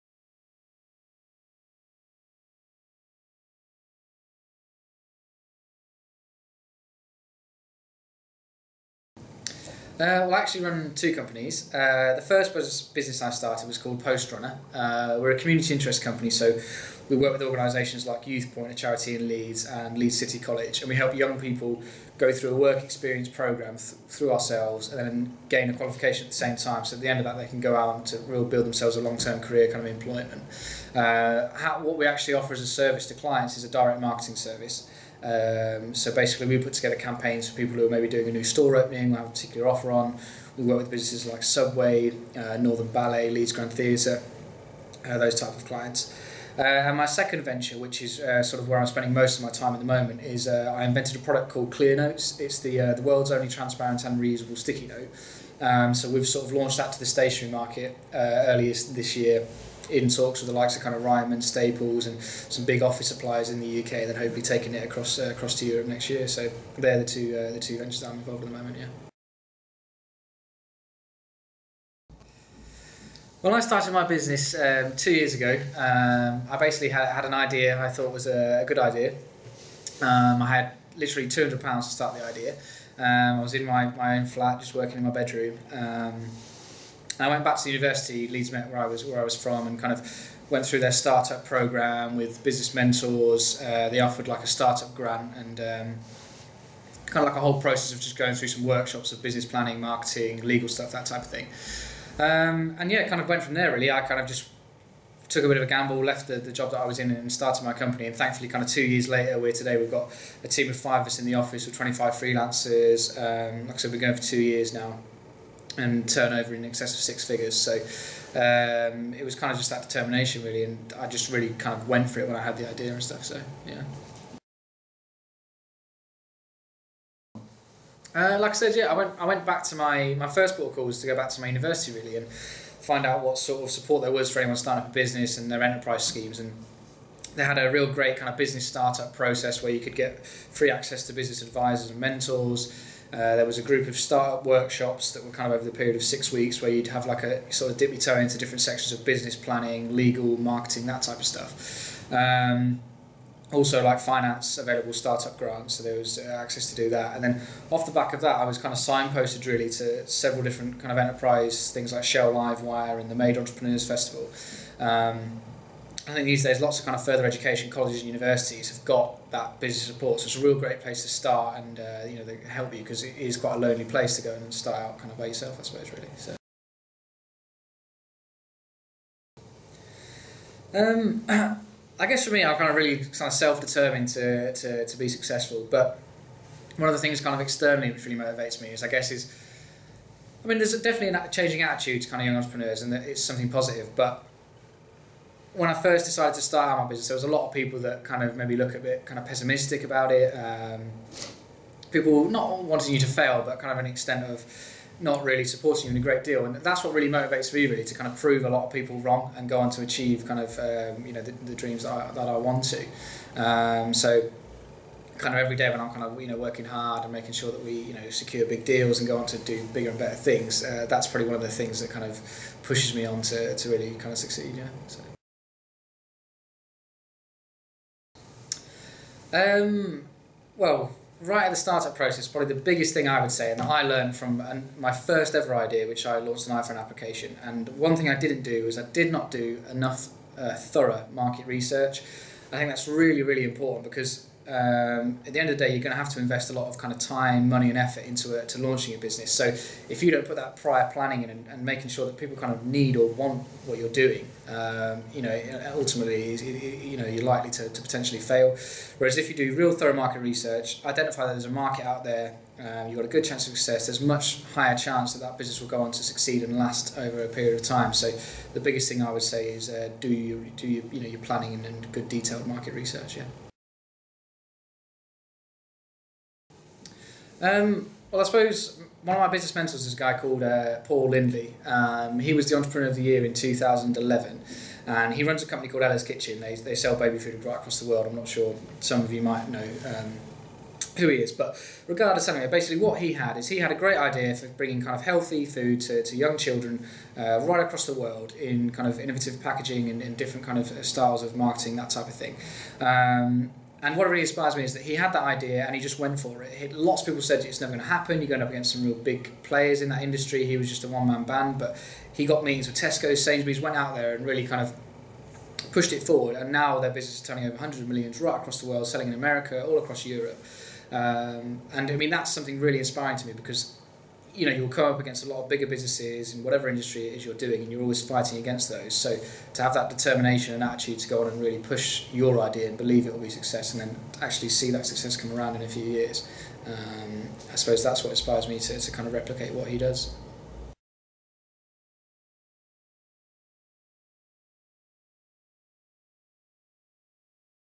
How I started my business... An Interview with John Macleod
An interview with John MacLeod, creator of Clear Note and Post Runner on how he started in business.